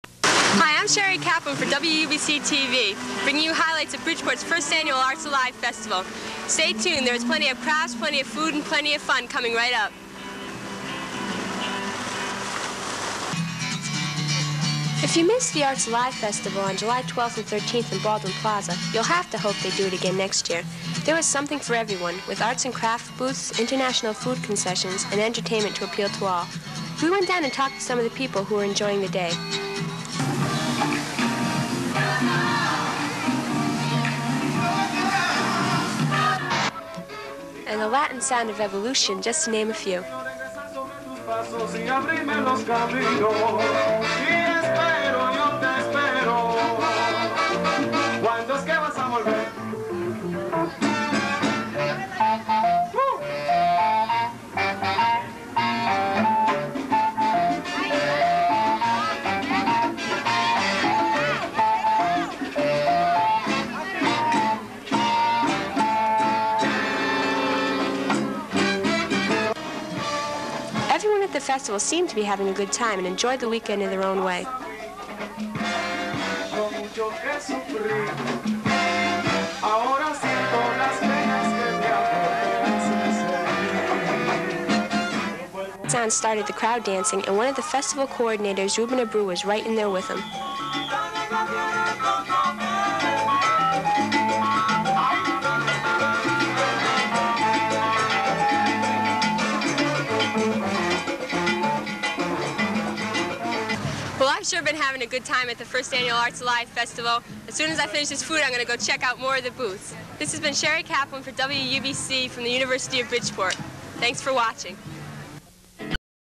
Orquesta Evolucion: Bpt Ct Arts Festival 1982
Edwin Rivera Singer, Nelson Rivera Bass & Coro, Rafael Rosado on Piano,
Louie Carrasquillo Conga, Robert Malines Timbale, Peter Bias Bongo, Peter Perez Trombone, Bill Trombone, Mark Richardson Trumpet, Edwin Olivieri Coro,